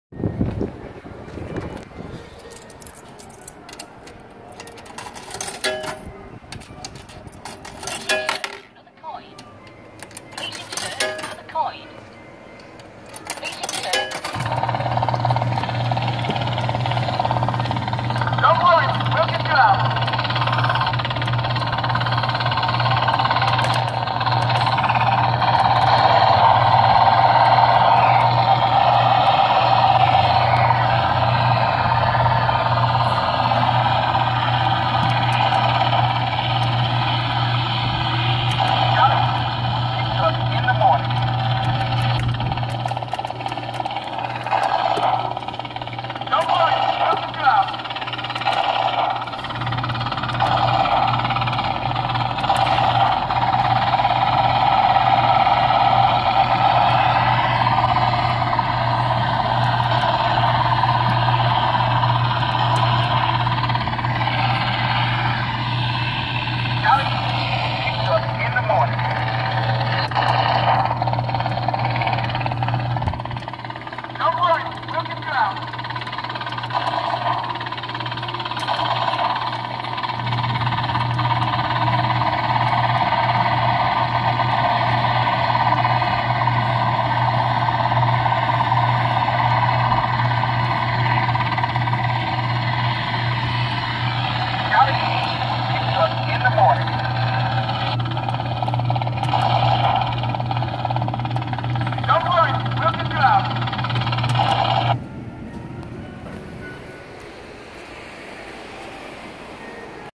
Jolly Roger Rescue Helicopter Kiddie Ride
Reupload from rescuehero942. Made and manufactured by Jolly Roger Ltd. in the 1990's Filmed at Sanibel Outlets in Sanibel, FL